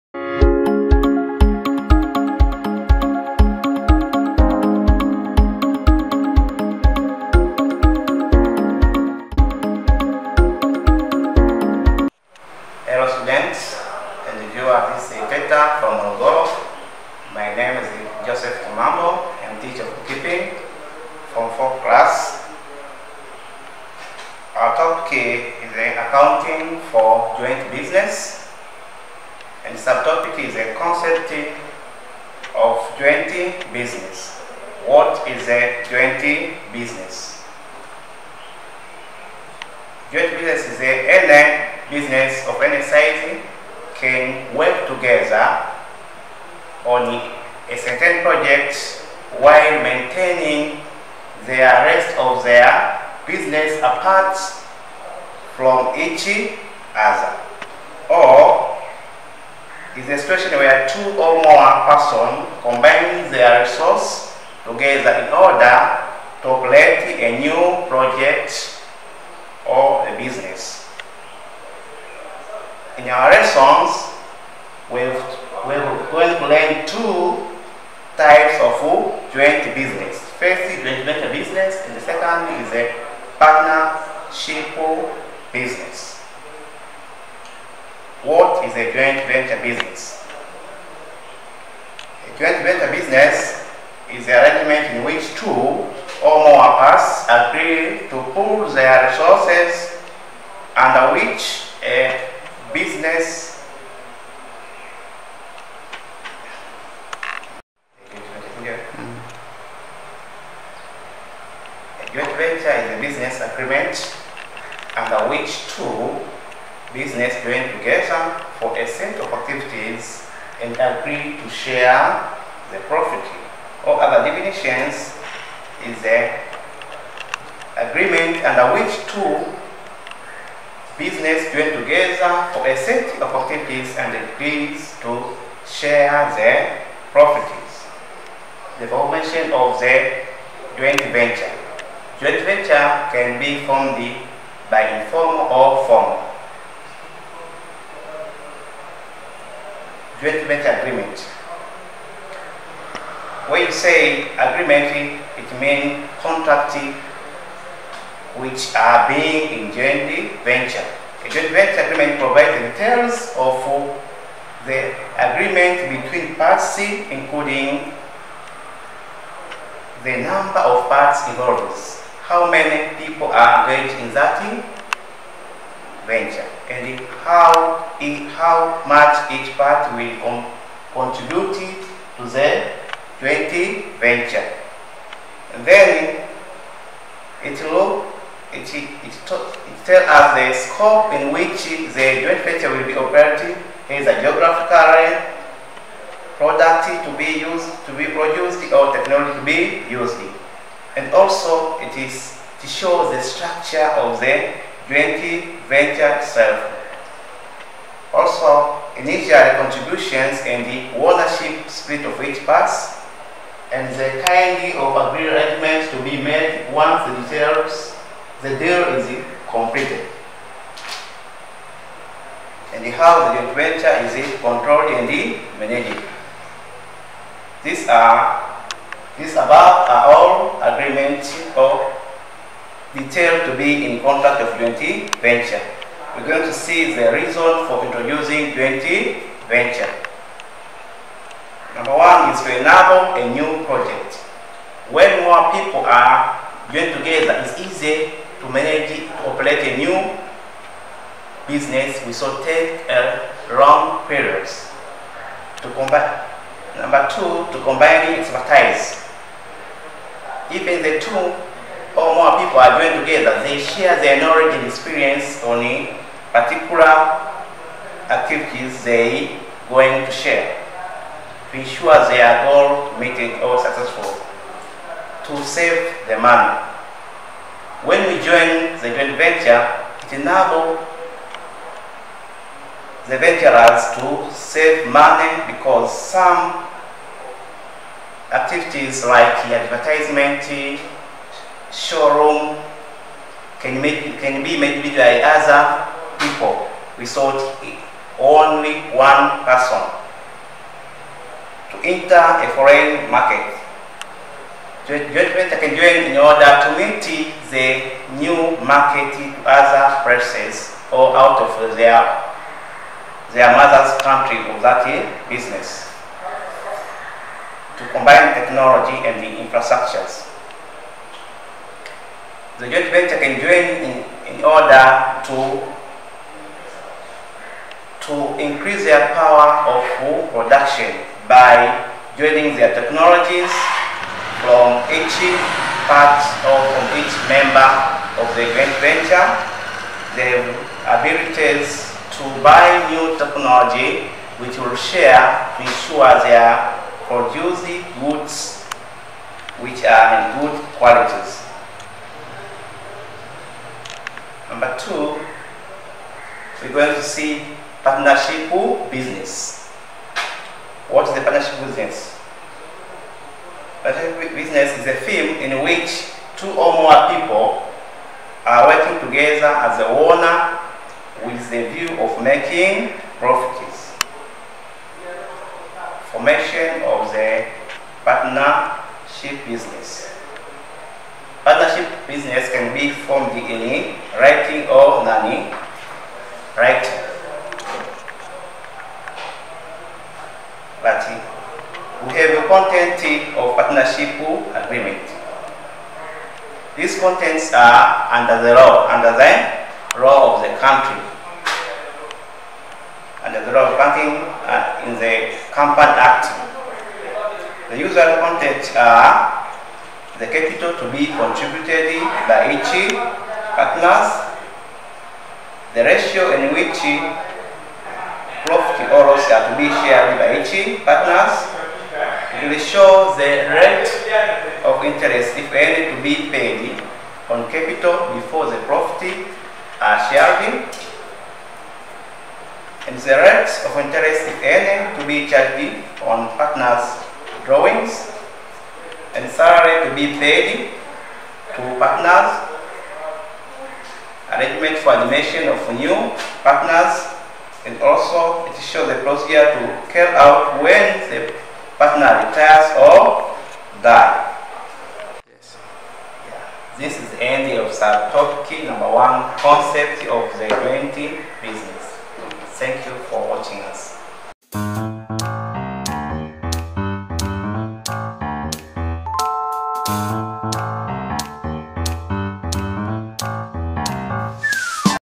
BOOK KEEPING FORM IV TOPIC: ACCOUNTING SUB TOPIC: CONCEPT OF JOINT BUSINESS
TEACHER'S NAME: JOSEPH KIMAMBO
IDARA YA ELIMU MKOA WA MOROGORO